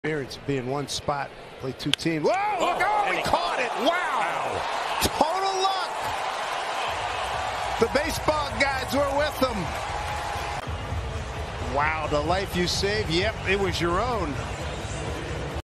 LOGAN LUCKY! Logan Gilbert SNAGS 106.4 MPH liner! 😱
Logan Gilbert's quick reflects.